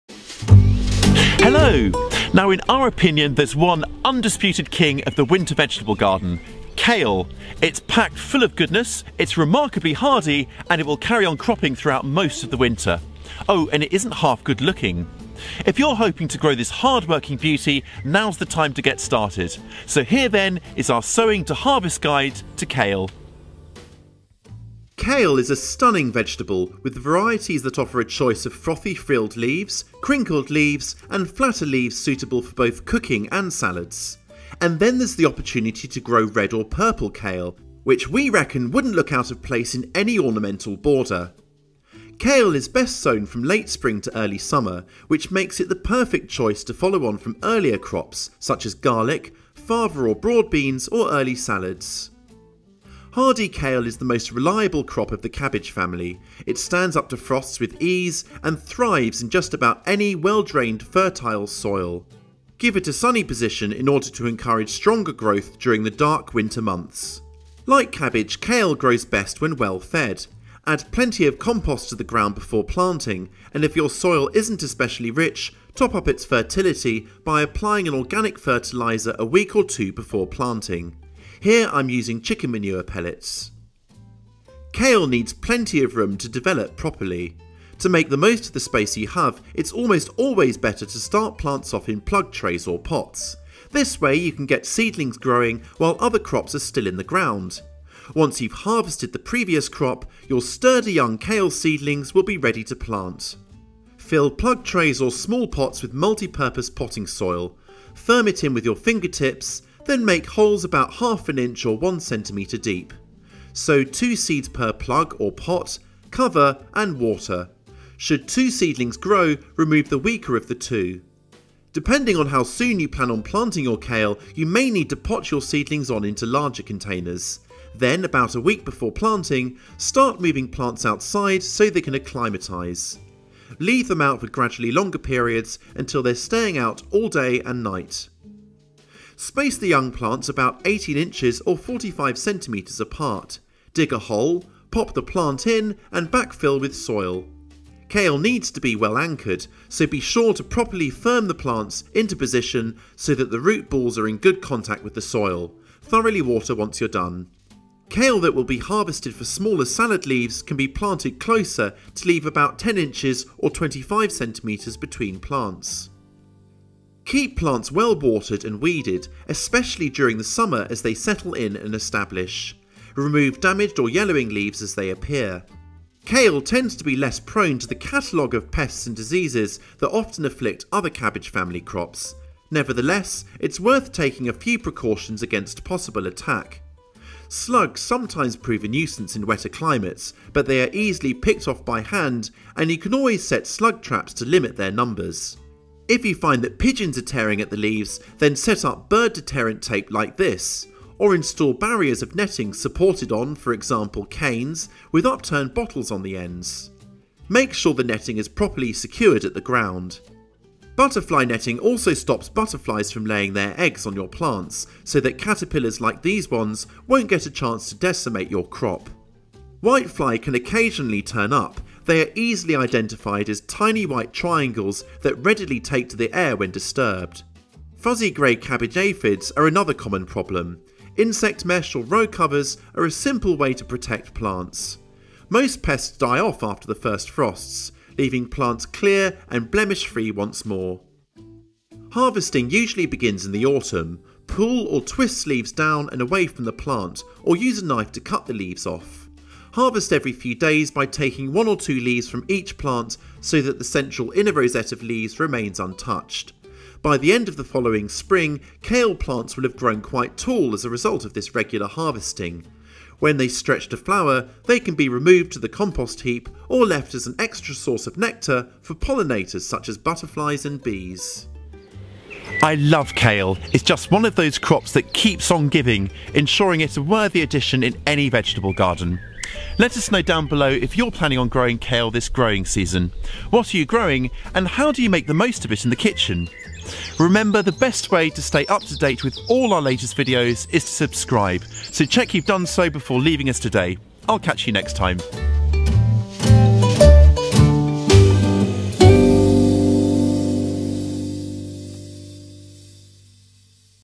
Growing Kale from Sowing to Harvest
Kale is the undisputed king of the winter vegetable garden! Nutrient-packed and remarkably hardy, it’ll carry on cropping throughout the coldest months and on into spring.

The best-looking of the brassicas, kale is a valuable addition to a potager or ornamental border as well as vegetable gardens.

In this short video we’ll shown you how to sow, plant, care for and harvest this stunning vegetable.